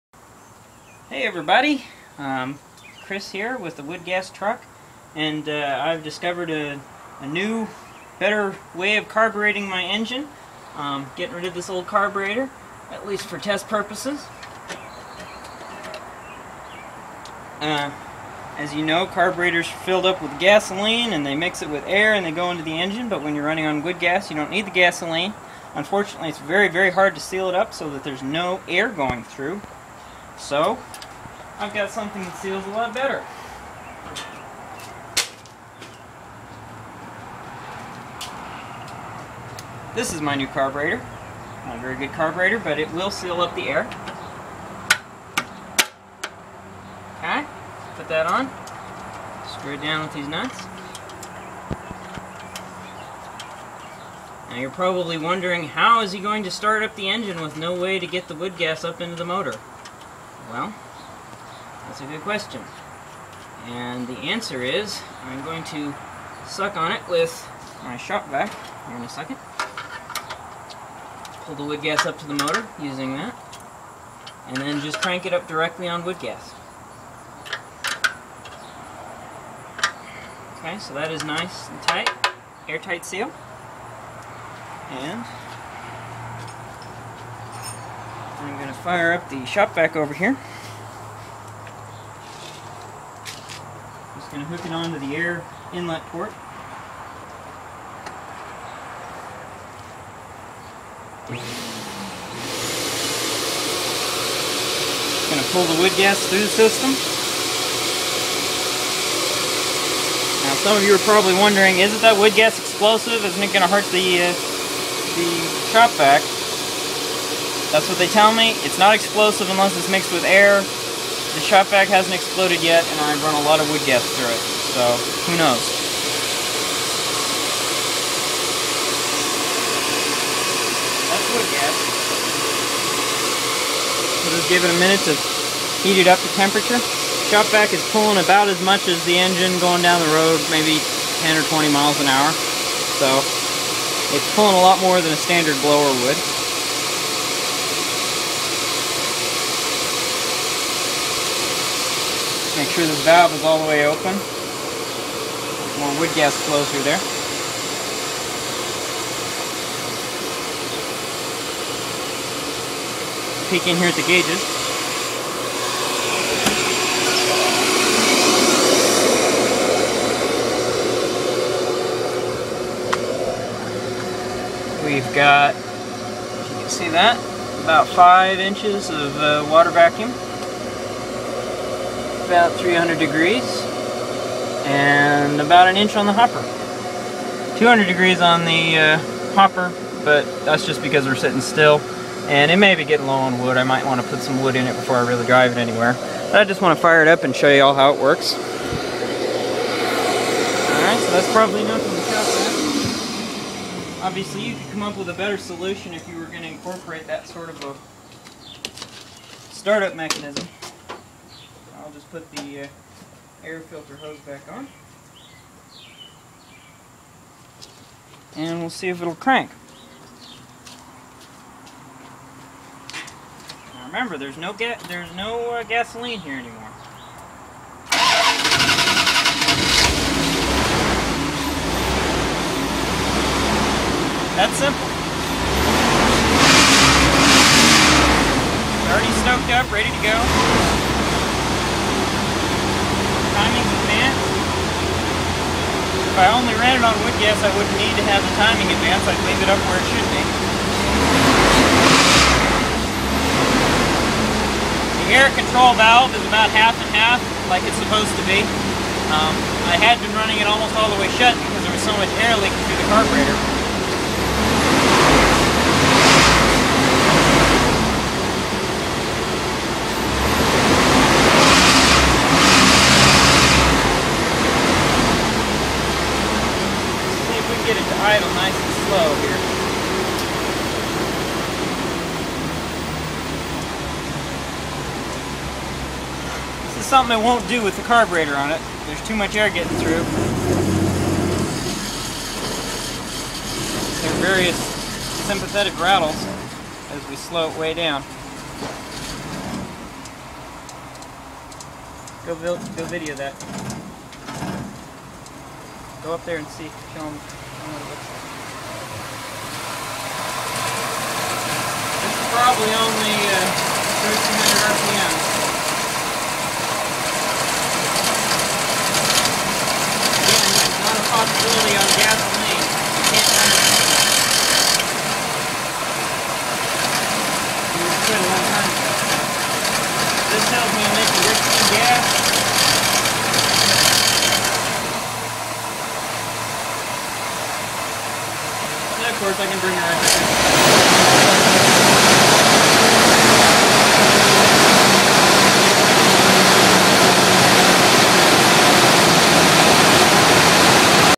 Look Ma, no carburetor!!
Chris takes the carburetor off his wood powered truck and bolts on a plastic cover instead. The engine still starts and drives great, better than ever - it loves the woodgas.